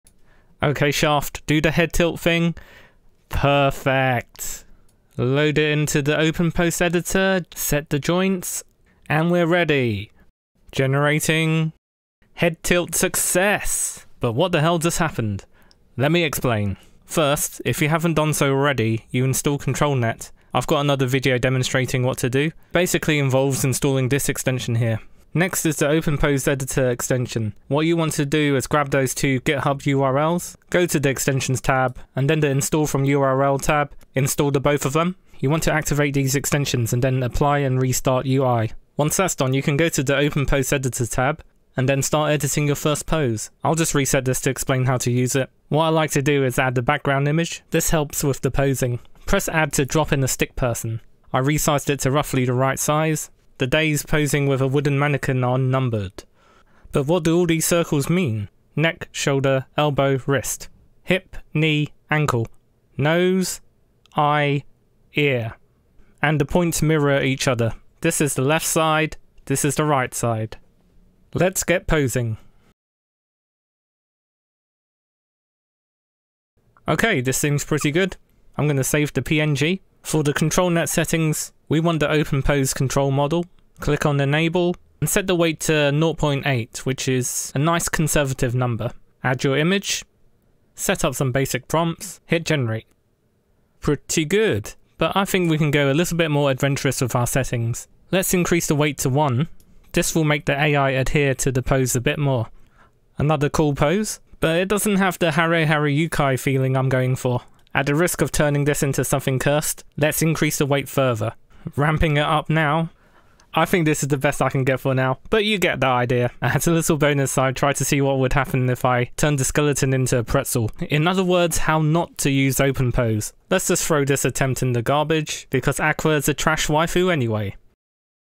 OpenPose Tutorial: Be a Poser
How to use ControlNet with the OpenPose Editor extension. Featuring Hitagi Senjougahara doing the famous Shaft studio head tilt, from the anime Bakemonogatari.

We explore what the joints mean and how to align them with background guide images to finely control the posing of your AI art characters.

The Automatic1111 WebUI is used for this Stable Diffusion guide. The OpenPose model is the cut down fp-16 safetensor version in

Also surprise appearance from Konosuba at the end.

Model used is AbyssOrangeMix2_sfw.